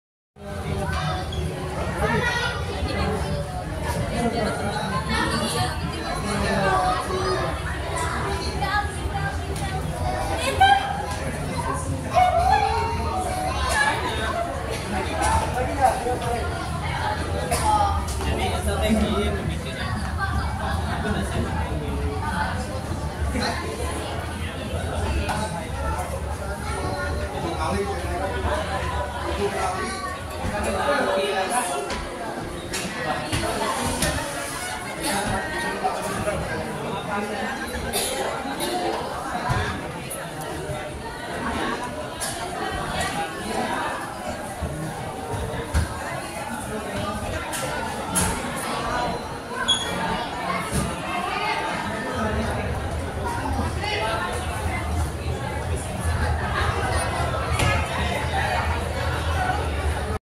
DISPLAY SCALE 1:1 :OLD CRAFTS at Geology Museum
Meet another to ‎@displayinAPAAJA   at scale 1:1 :
Found Old Crafts ( Part 25 ) at Geology Museum ( Bandung - West Java , Indonesia ) ..